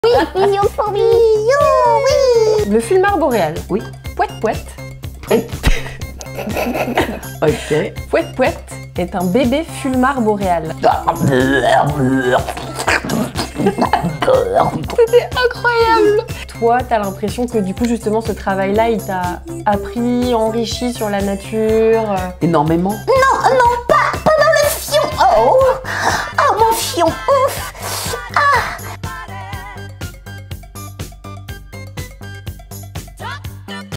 Brigitte Lecordier imite des animaux
Nouvelle vidéo sur ma chaîne ! J’ai lancé un défi à @BrigitteLecordierYT et on a bien rigolé 😅